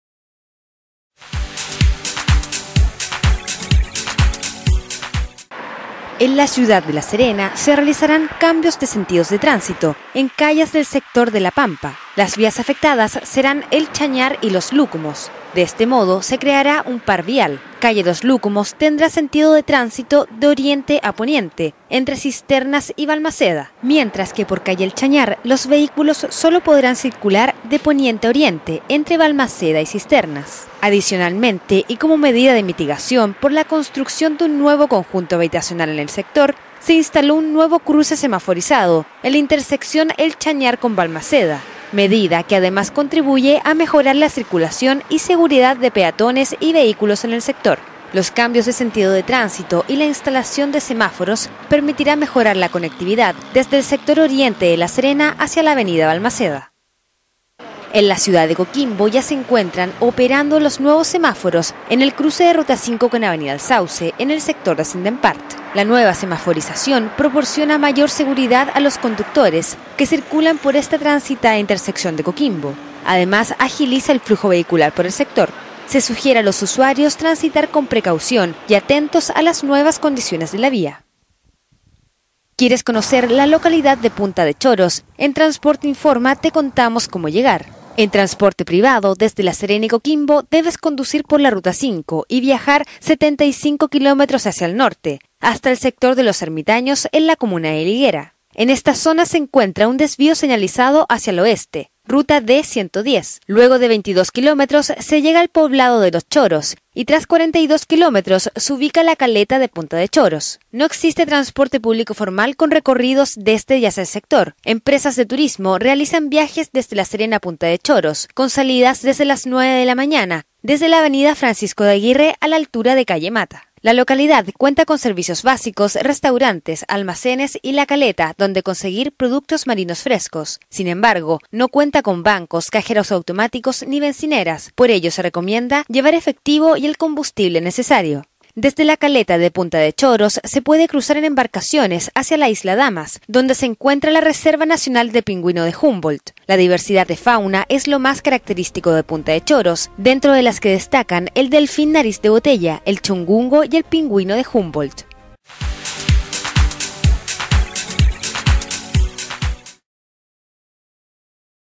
Boletín de Noticias Semanal del 13 al 20 de febrero
Conoce los principales eventos viales que afectan la movilidad en la Región de Coquimbo y las rutas para acceder a los destinos turísticos. Muévete informado con transporTeinforma.